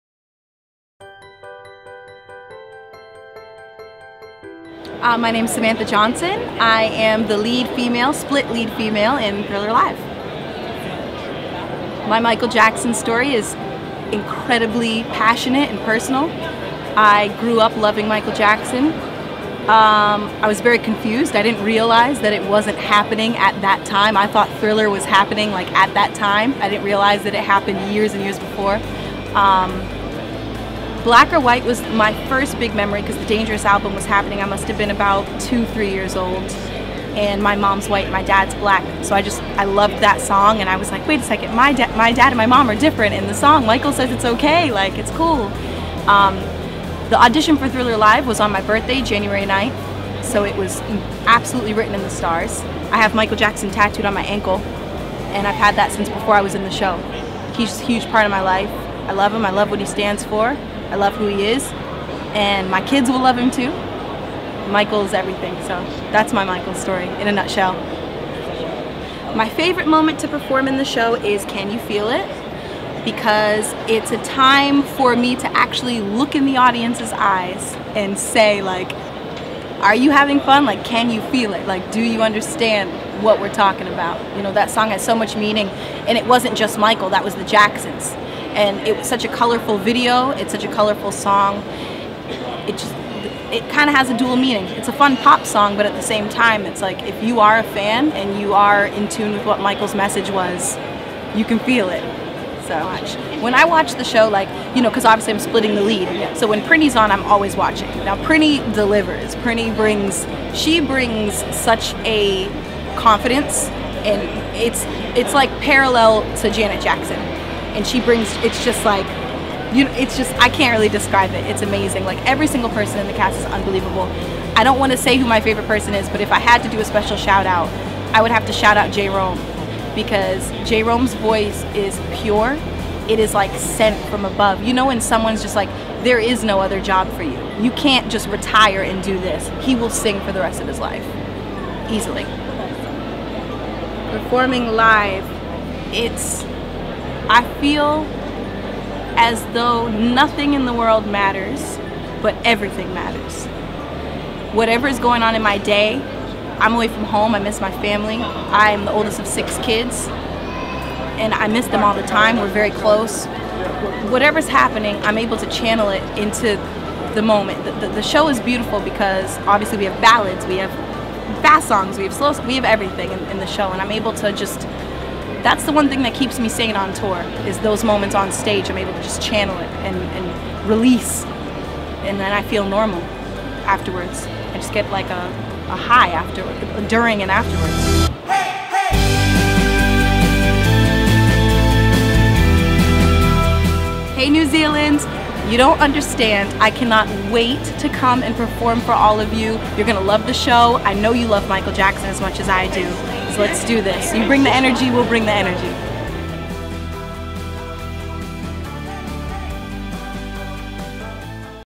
Meet THRILLER LIVE's Samantha Johnson
THRILLER LIVE comes to The Civic, Auckland from 12-22 February! We caught up with Samantha Johnson after the show had its down under debut in Perth.